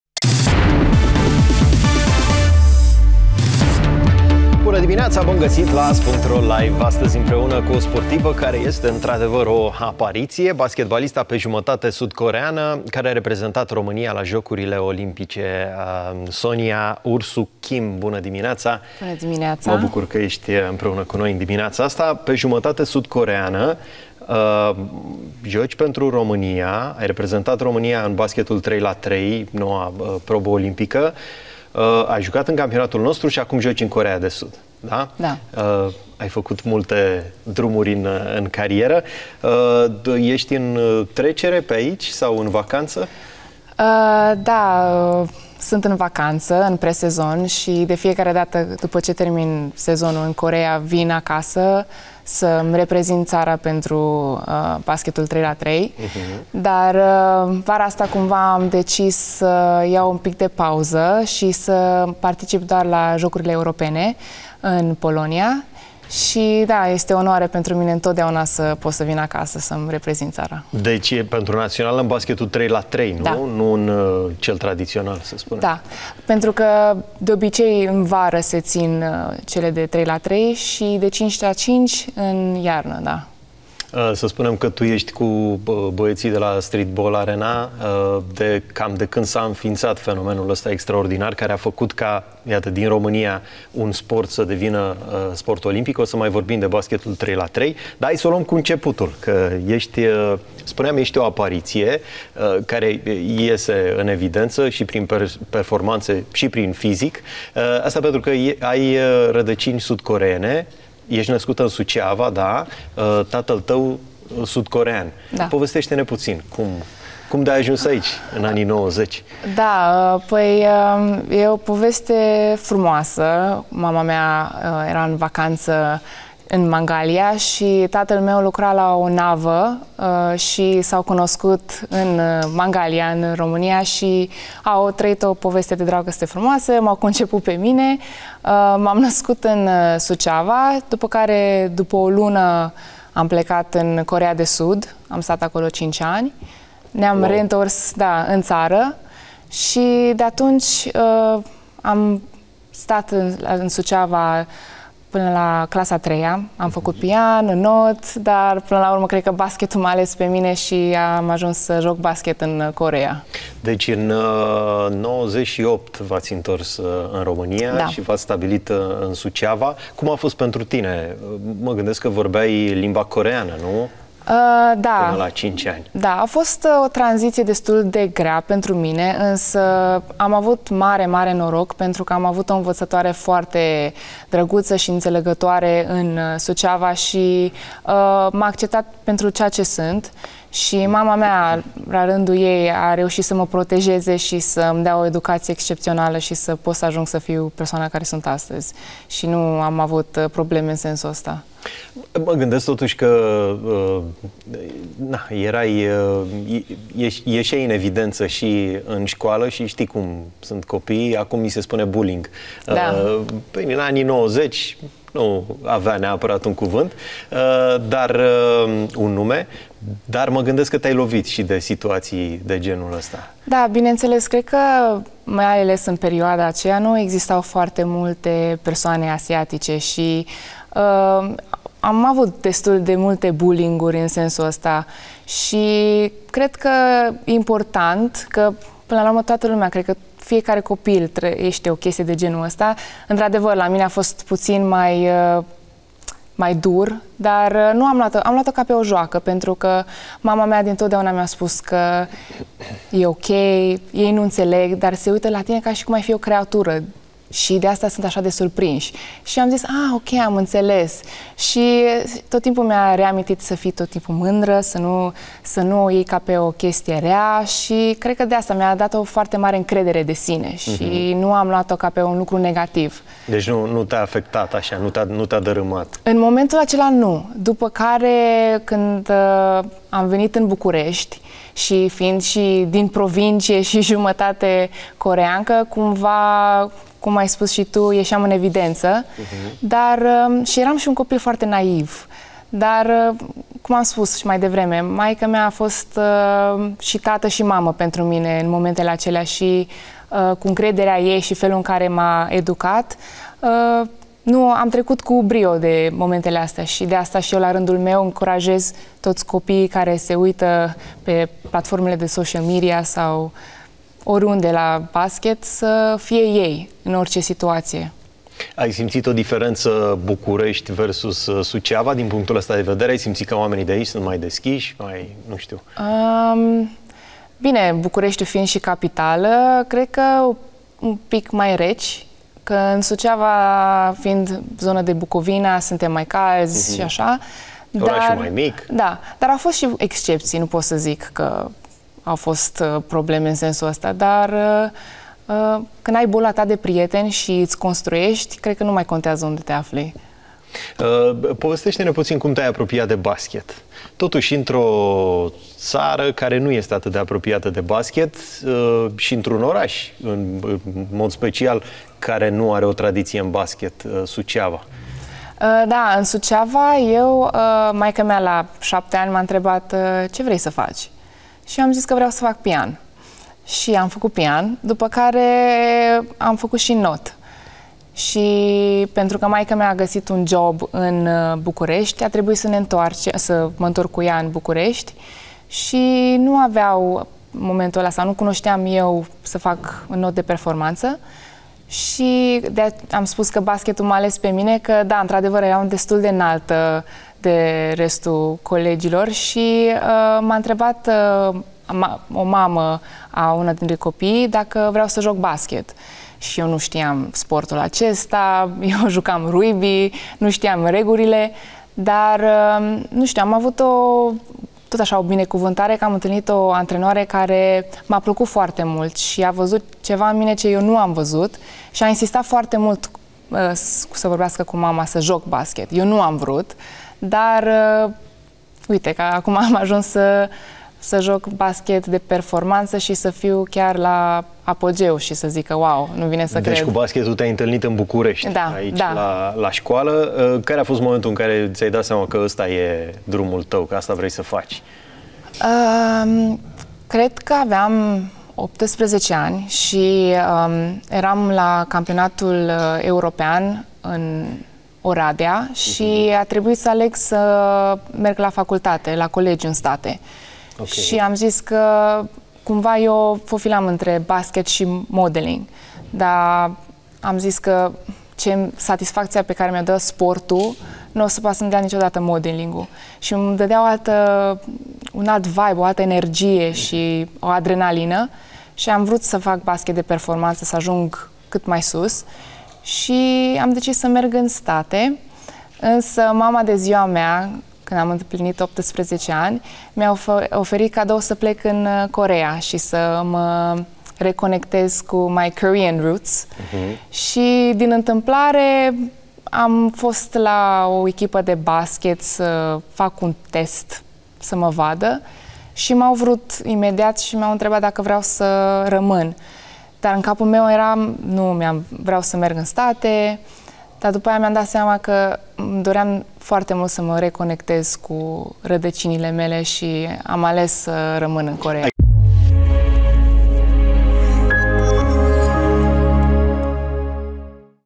AS.ro LIVE | Sonia Ursu-Kim la AS.ro LIVE, partea 1. ”Este o onoare să joc pentru România”
Sonia Ursu-Kim s-a născut în Suceava, dar şi-a petrecut primii ani din viaţă în ţara natală a tatălui ei, Coreea de Sud. A început să joace baschet la 11 ani şi a câştigat, cu echipa României, Europeanul U18 din Divizia B.